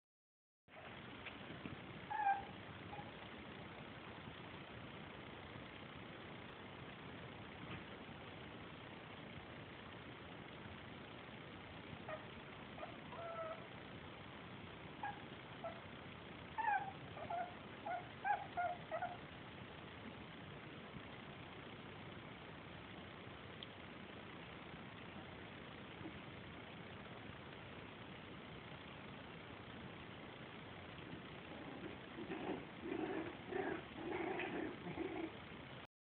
Bébés bichons havanais, un mois, jouent ensemble
Bébés bichons havanais, un mois. Les deux mâles jouent.